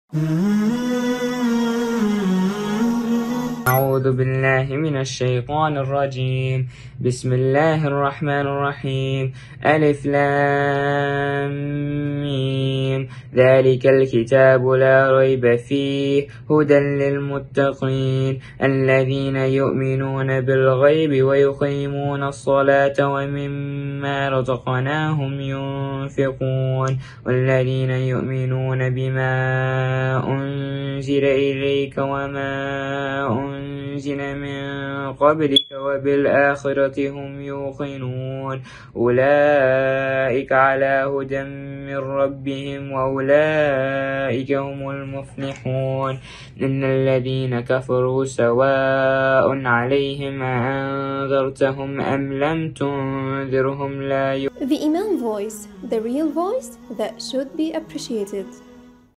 Feel the Passion: Mounir Khalil's Soulful Performance in Imaam Voice Competition
"Witness the soulful performance of Mounir Khalil, participant no. 50, in the Imaam Voice Competition. Mounir's powerful and emotive voice truly captivates the audience, showcasing his immense talent and dedication to music. Celebrate his journey and experience the passion he brings to the stage.